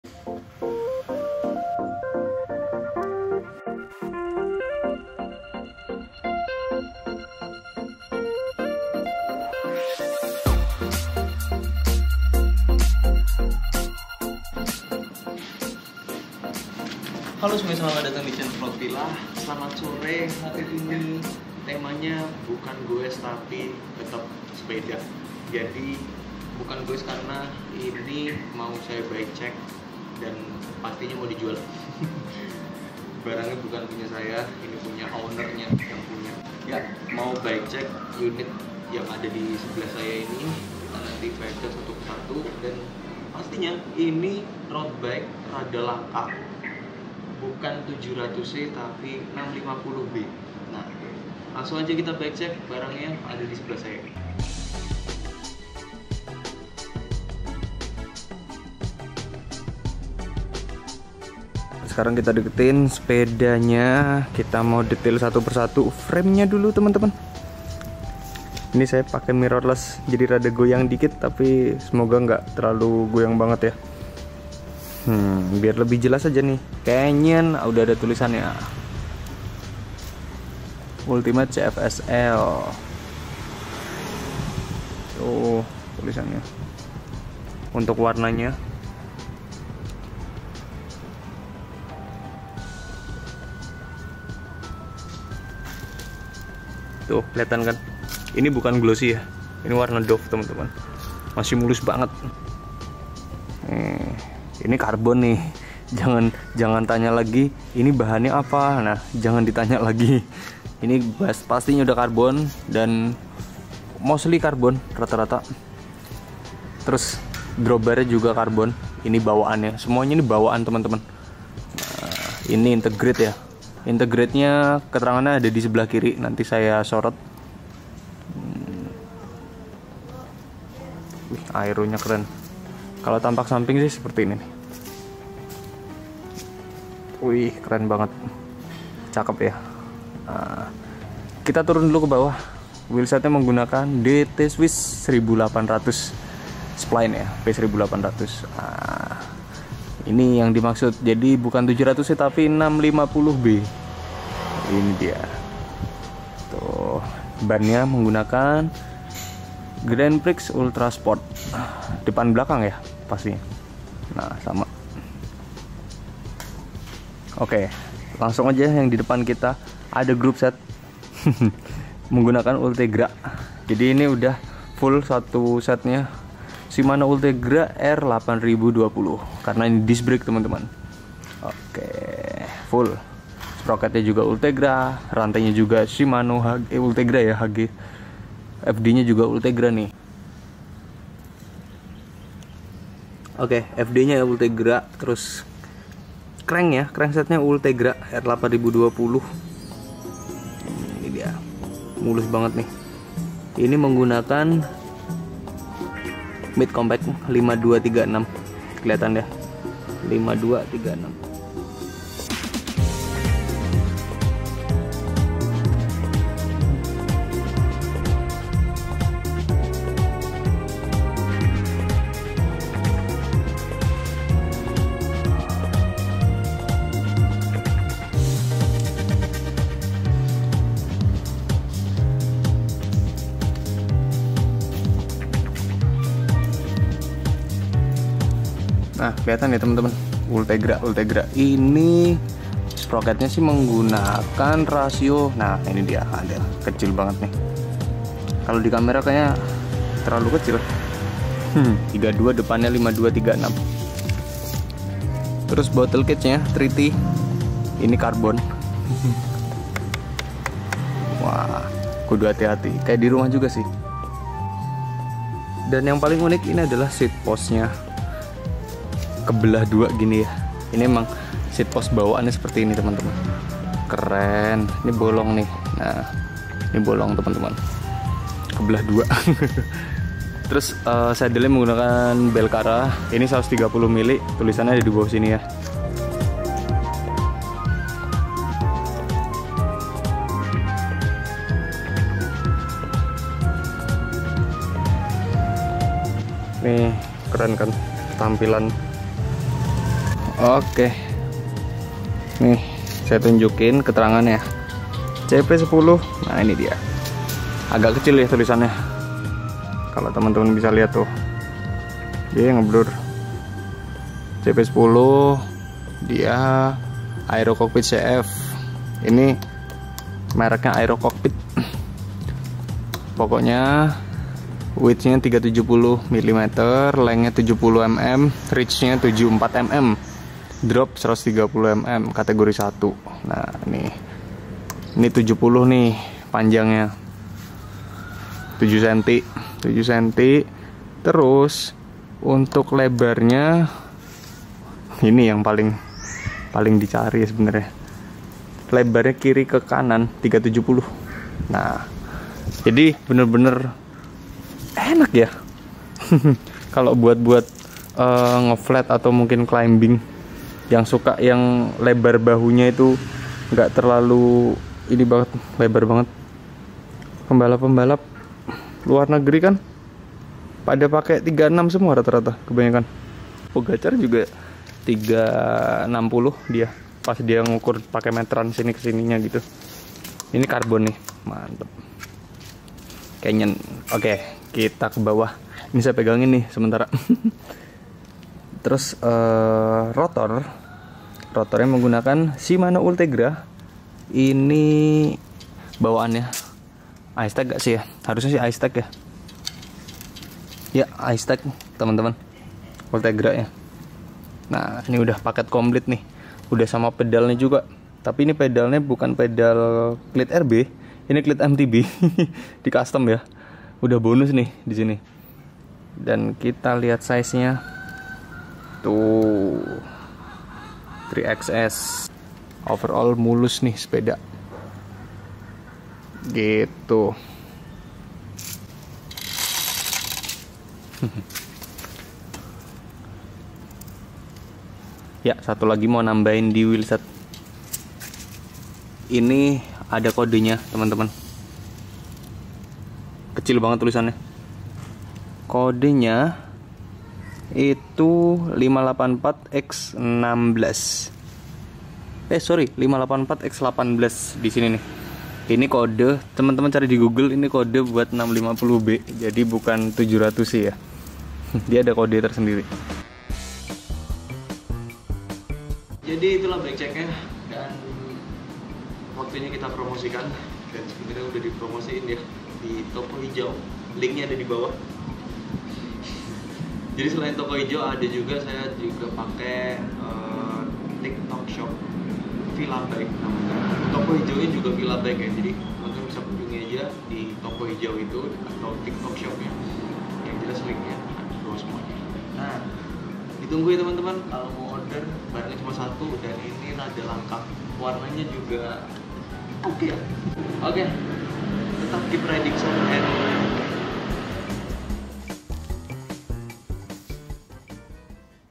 BIKE CHECK FOR SALE, CANYON ULTIMATE CF SL
Sepeda ini di cek spesifikasinya untuk di jual